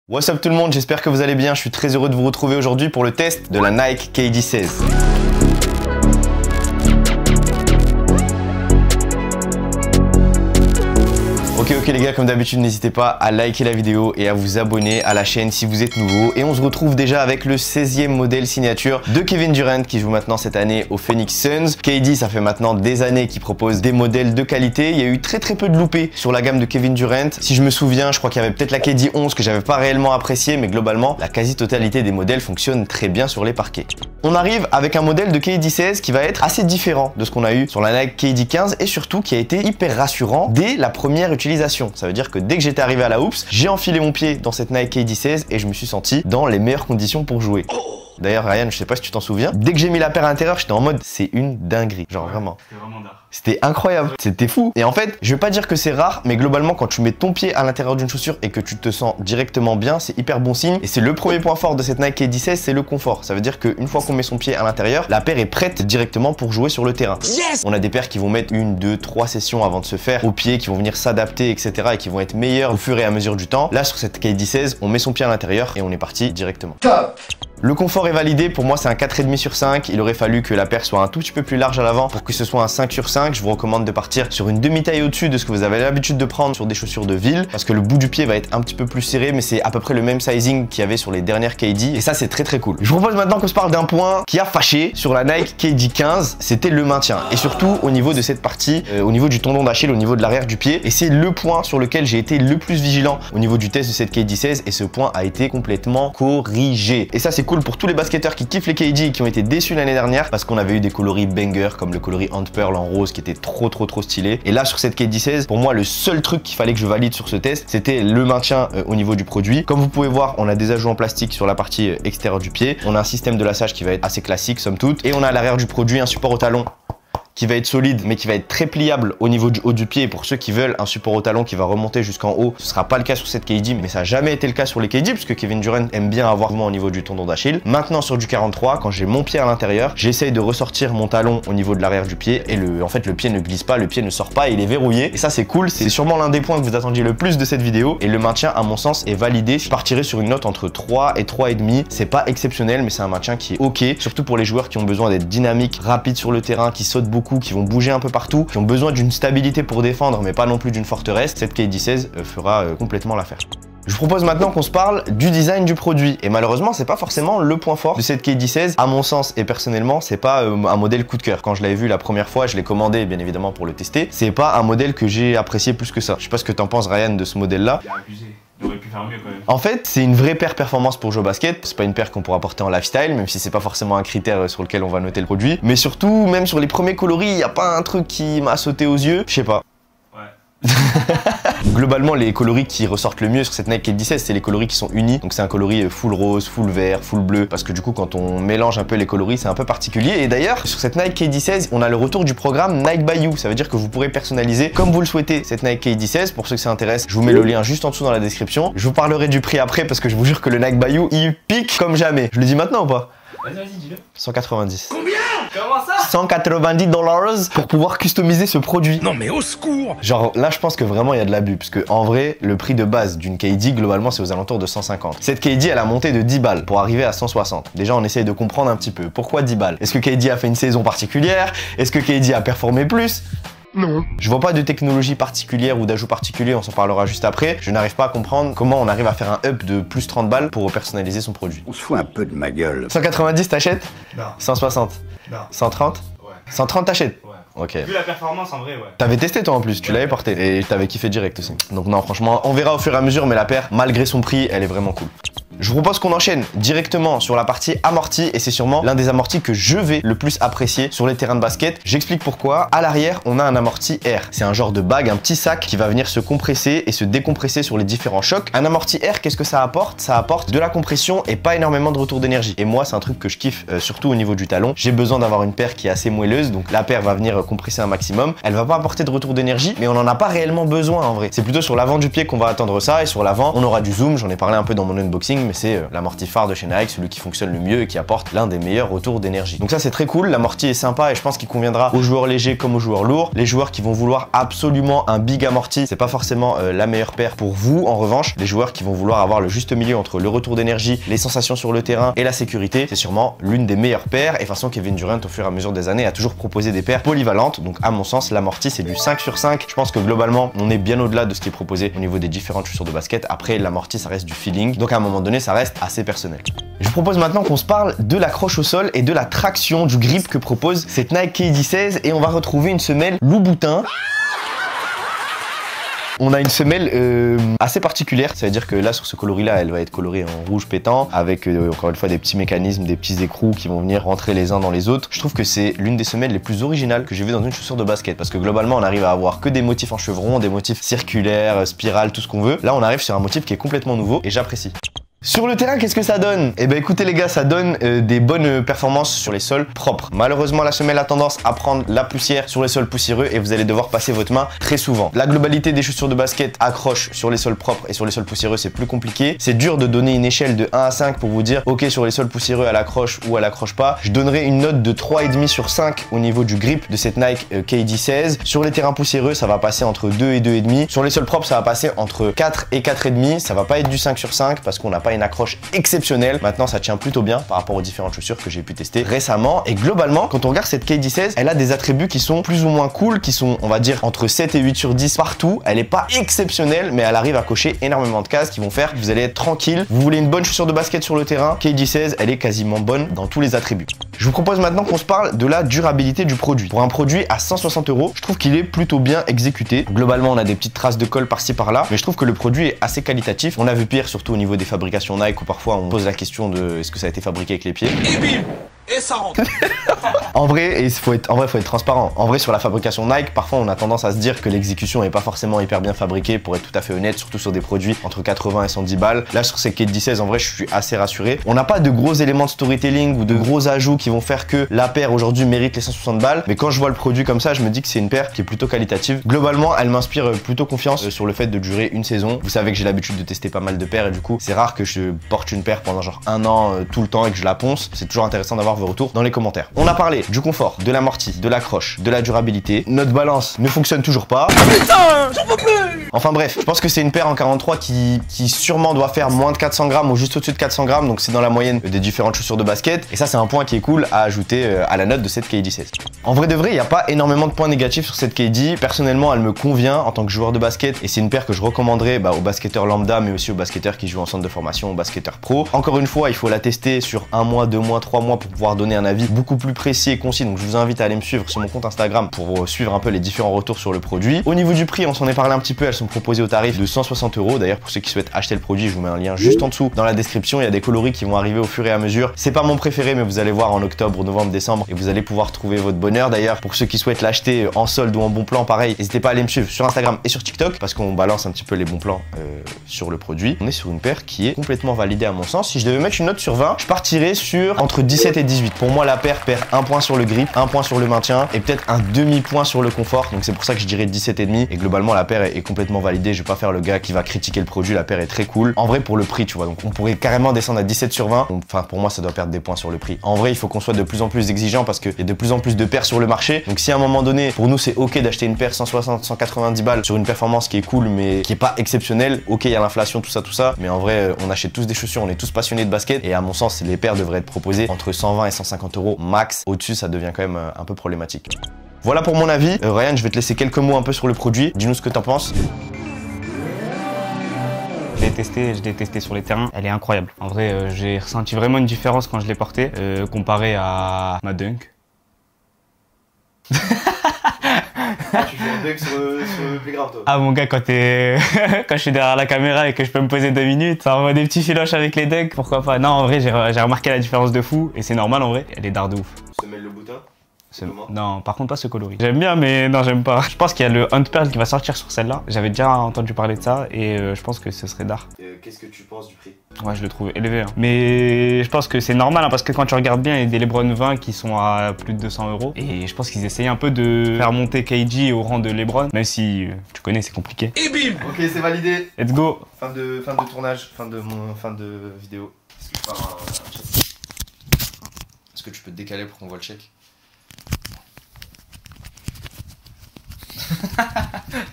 NIKE KD 16 TEST: UNE PERFORMANCE TOP NIVEAU?
00:00 - Intro
00:35 - Premiers ressentis
01:01 - Confort
01:34 - Maintien
02:43 - Design
03:20 - Nike by You & avis
04:15 - Amorti
05:17 - Traction/grip
06:34 - Durabilité
07:19 - Poids
07:35 - Impressions globales & réflexion sur le prix
08:58 - L'avis de Samouraiyan
09:41 - Outro